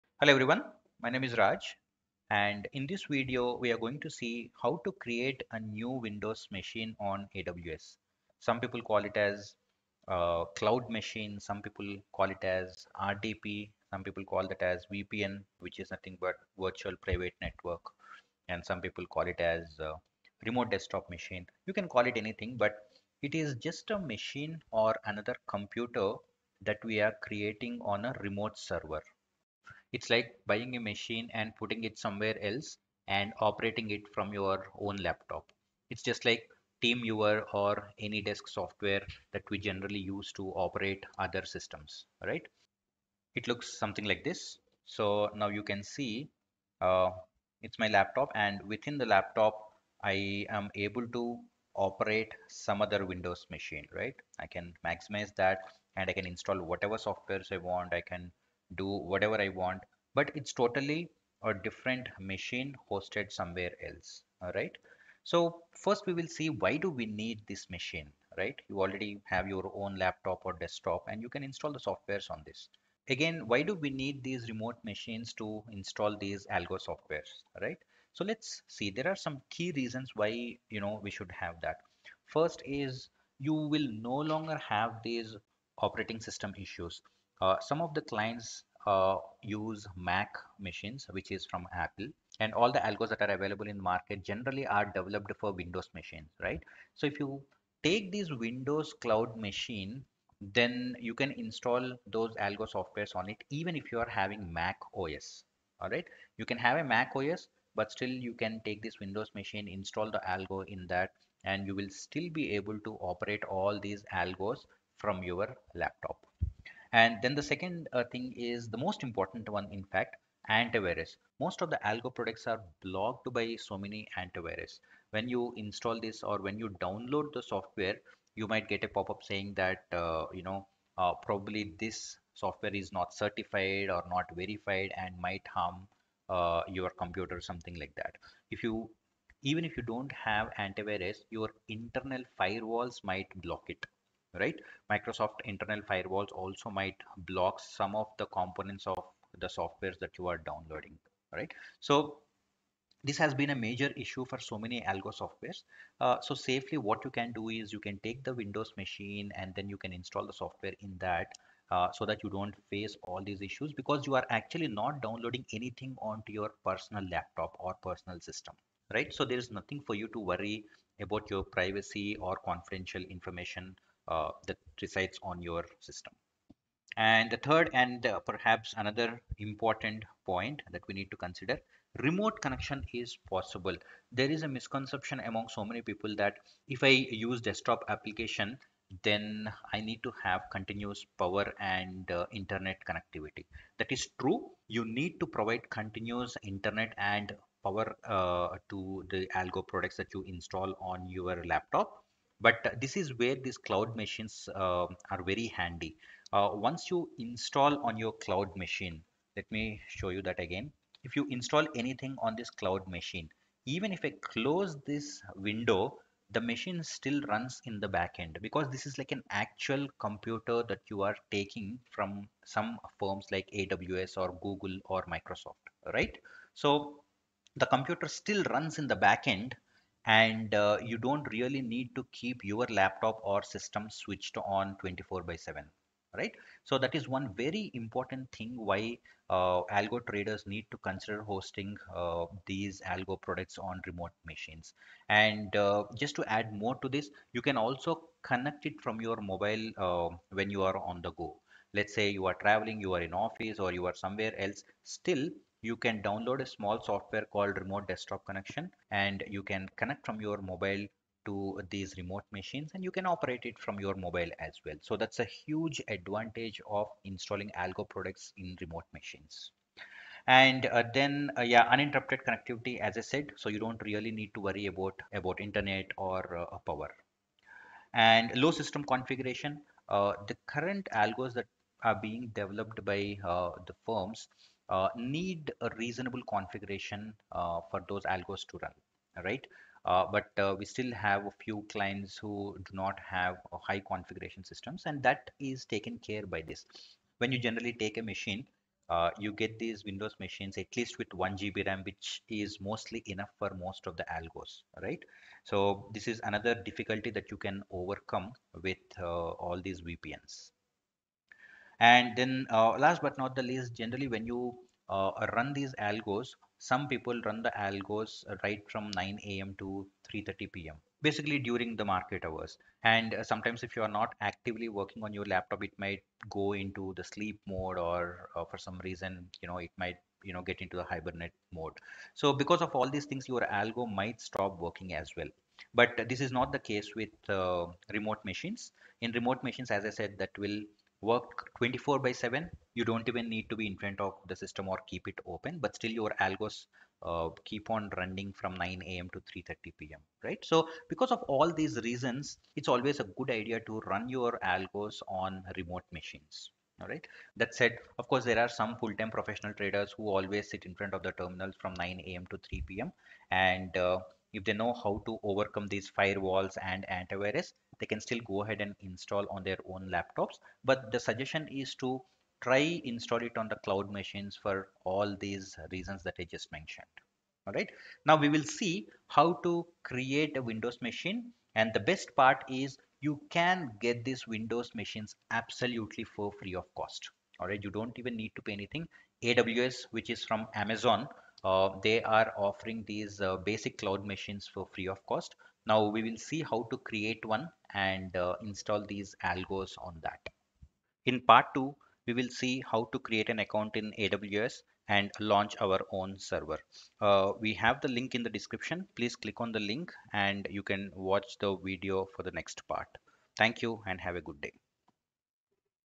Why do we need Free AWS server to install Algo Tools? - Part 1
In this video, we explain why it's important to have an AWS windows server (compared to our own laptop/computer) to install algo tools.